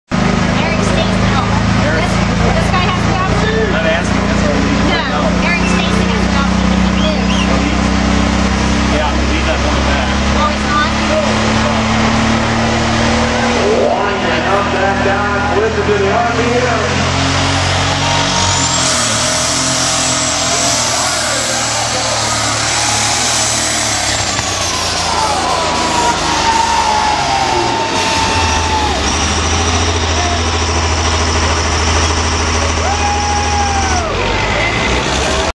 2011 WTF TODD GETTELFINGER
TODD GETTELFINGER AT 2011 WAPAK TUG FEST